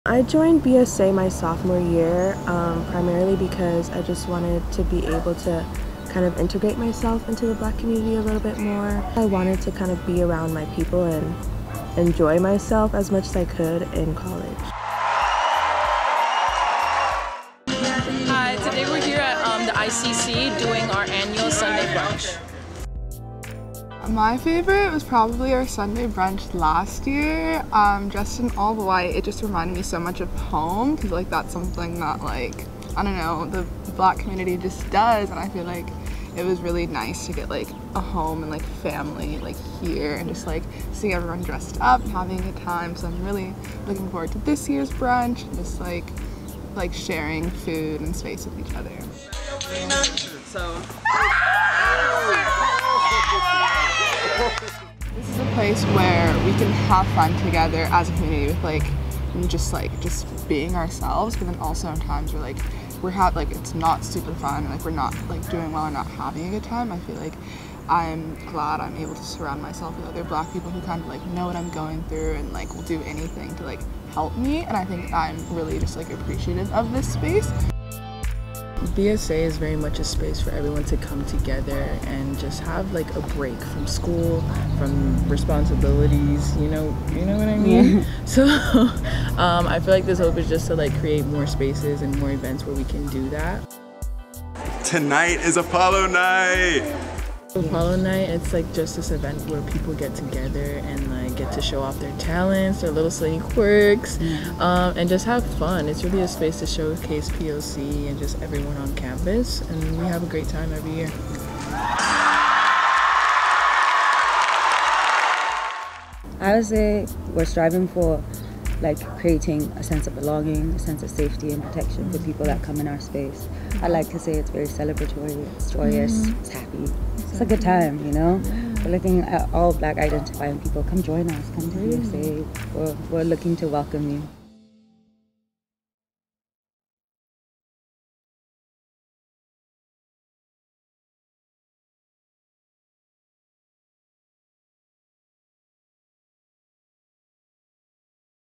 Meet the Black Student Alliance at Occidental College
Get to know the Black Student Alliance at Occidental College as they host a Sunday Brunch and their annual Apollo Night talent show. Members of the affinity group share what BSA means to them and what future students can find through the club.

Video by Maggie Frank '23 for the Occidental College Office of Admission